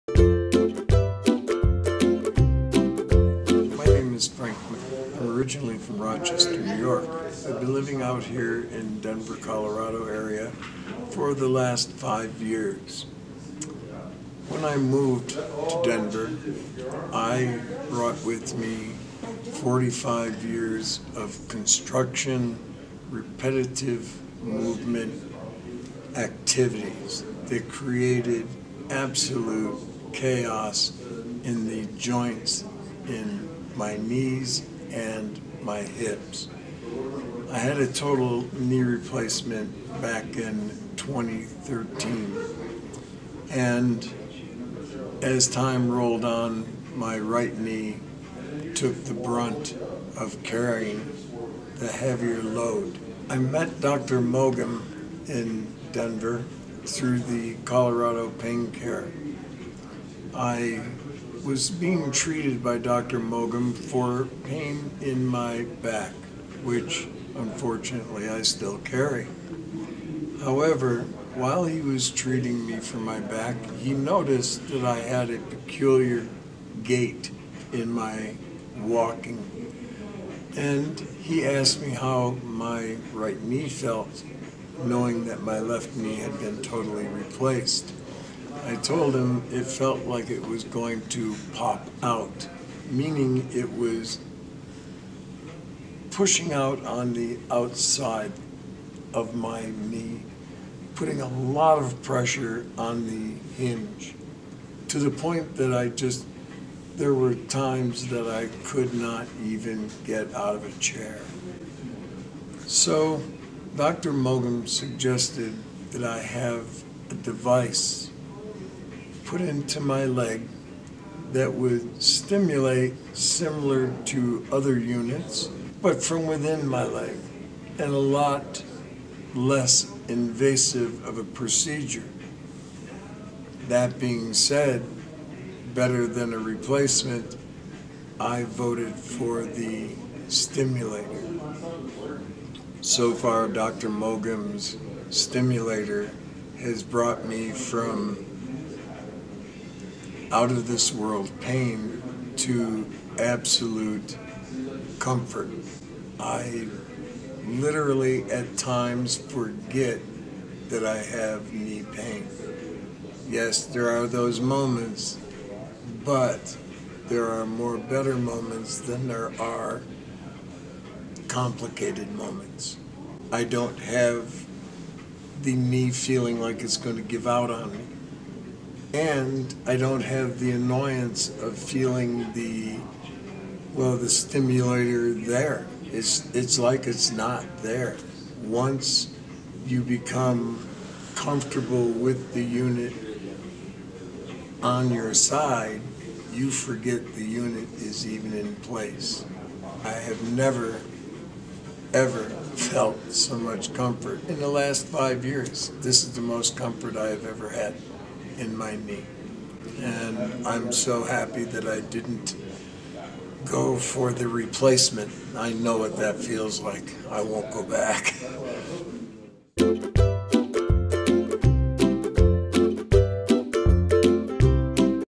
Colorado Pain Care Peripheral Nerve Stimulator Patient Testimonial
Check out what Frank M. has to say about his recent experience with CPC!